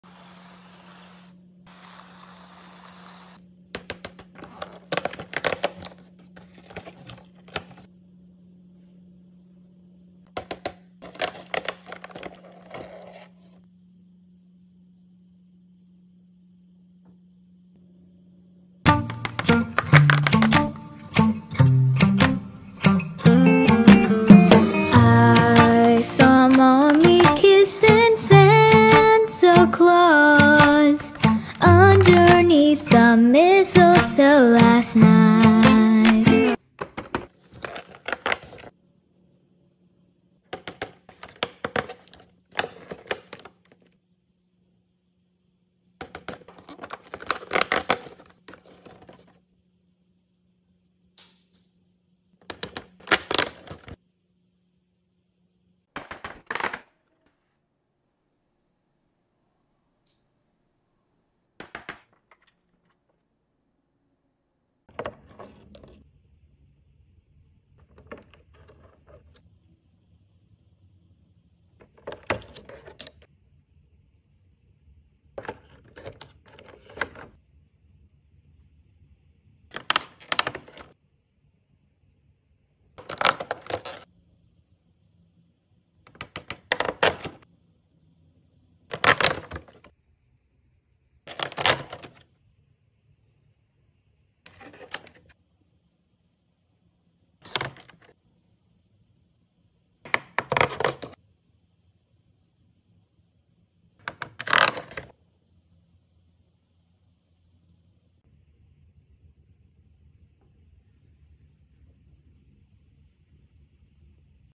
Miraculous Ladybug Advent Calendar ASMR Unboxing 🎄🎁 | 24 Miniature Surprise Figures
Opening Miraculous Kwami Advent Calendar with 24 surprises